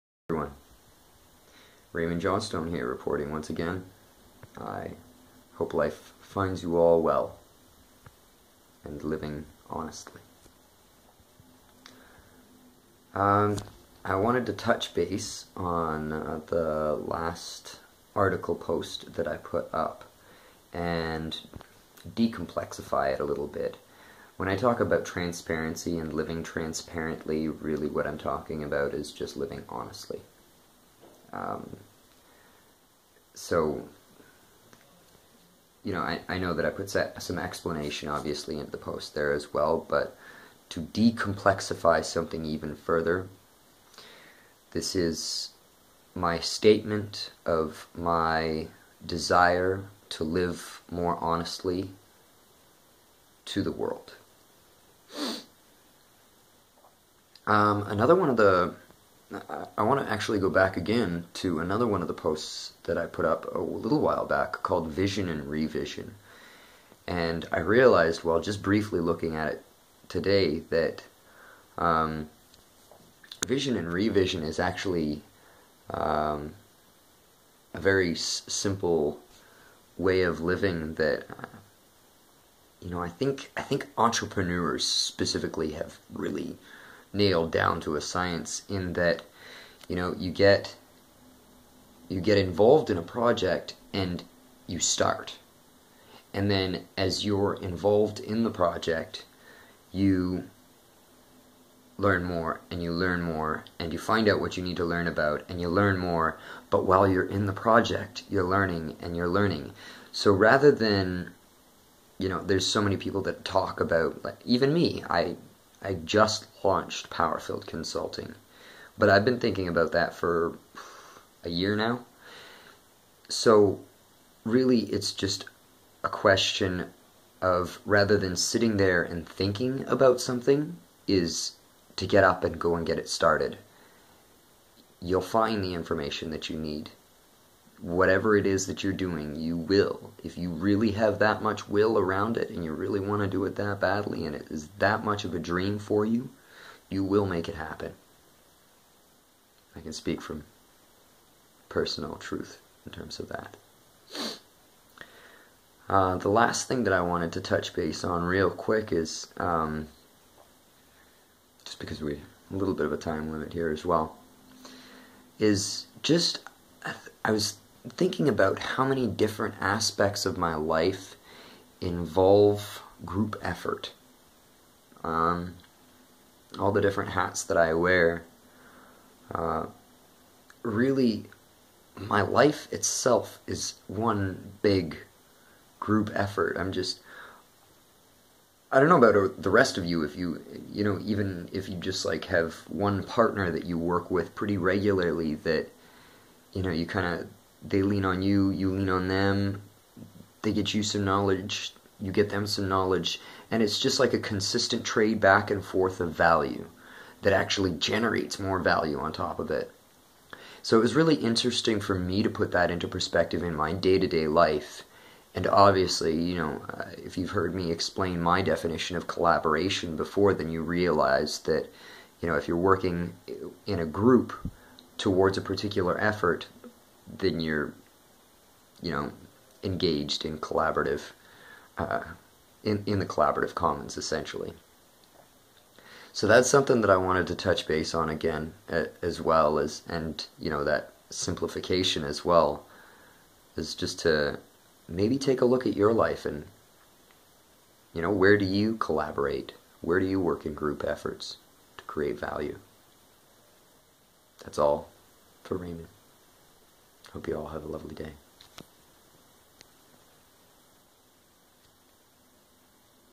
Old Notes
Raymon Johnstone talking about a couple different topics that he has covered before and building upon them some more.